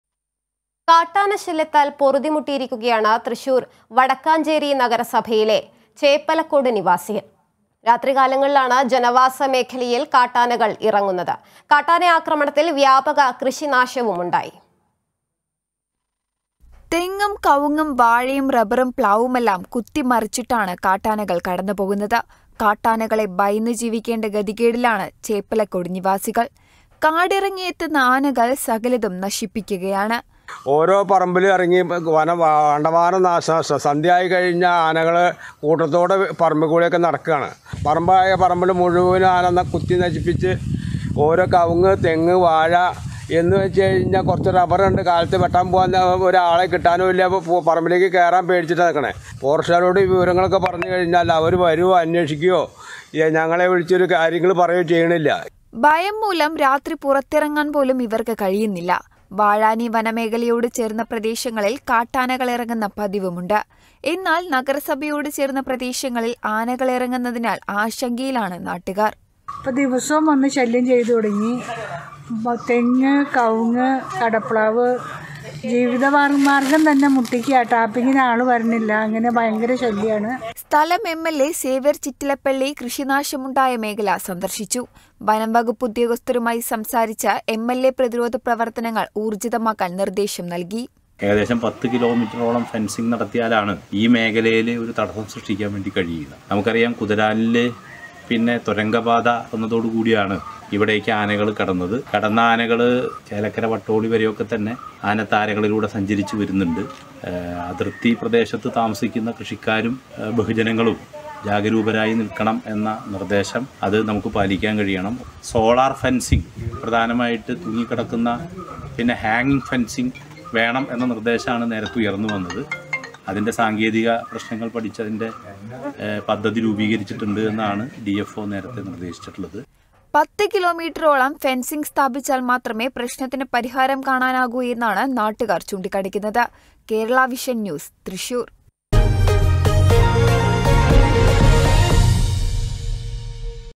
കാട്ടാന ശല്ല്യത്താൽ പൊറുതിമുട്ടി ചേപ്പലക്കോട് നിവാസികൾ
About COA